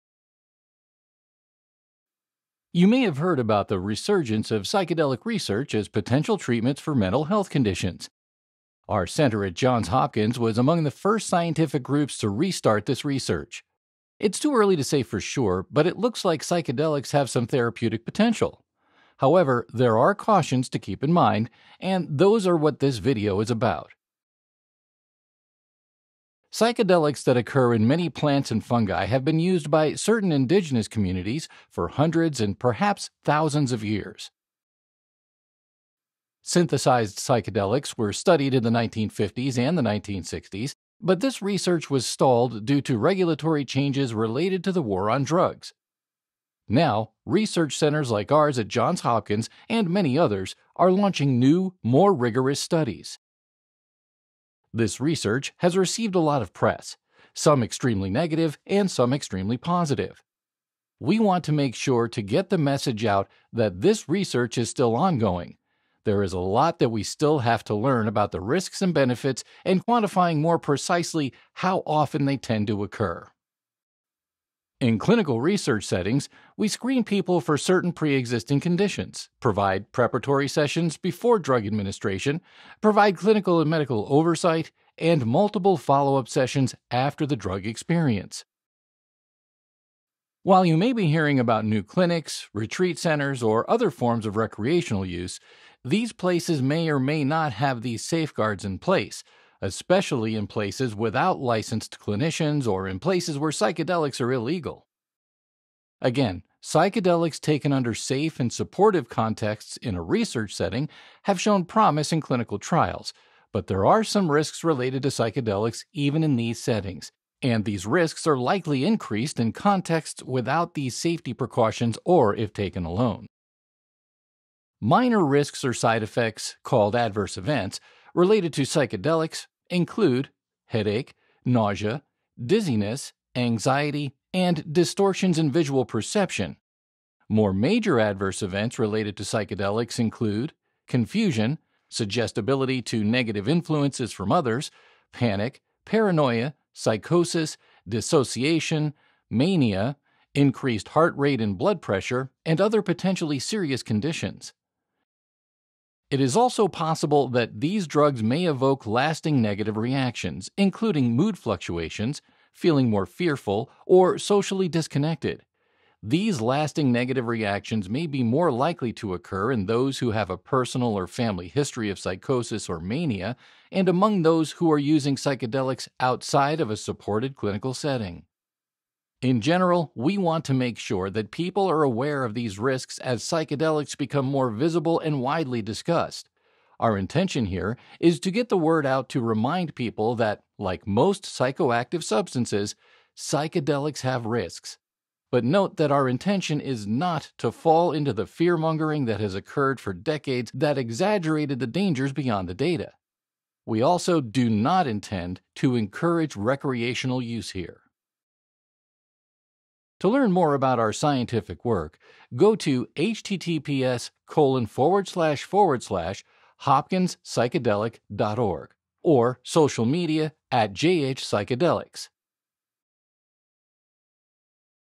JHMI - Psychedelic Risks, Side Effects, and Potential Harms
You may have heard about the resurgence of psychedelic research as potential treatments for mental health conditions. Johns Hopkins Center for Psychedelic & Consciousness Research was among the first scientific groups to restart this research. It's too early to say for sure, but it looks like psychedelics may have some therapeutic potential. However, there are cautions to keep in mind. We want to make sure that people are aware of these risks as psychedelics become more visible and widely discussed.